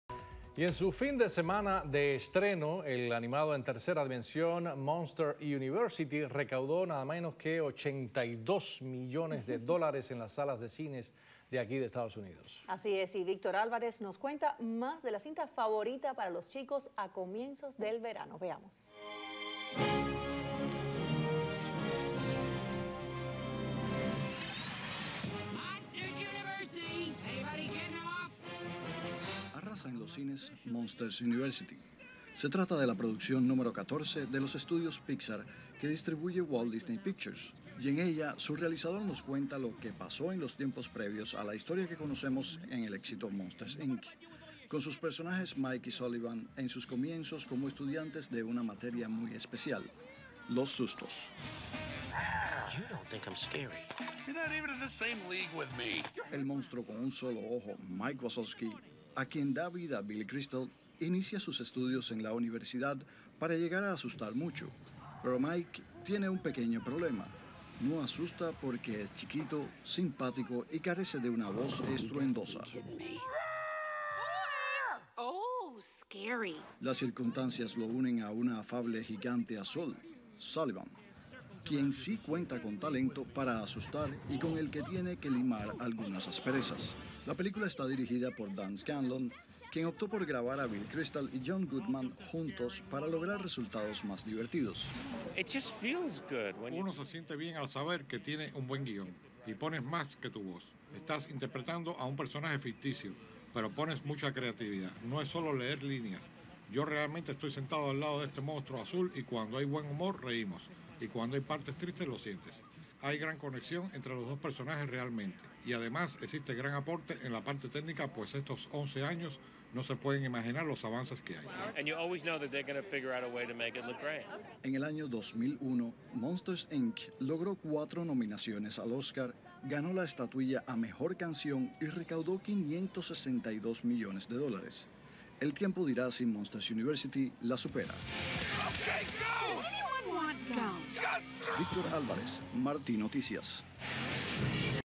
Arrasa en los cines Monster University
Junio 25-Una cinta del gusto de grandes y chicos, Monster University, es éxito de taquilla. Victor Alvarez con el informe.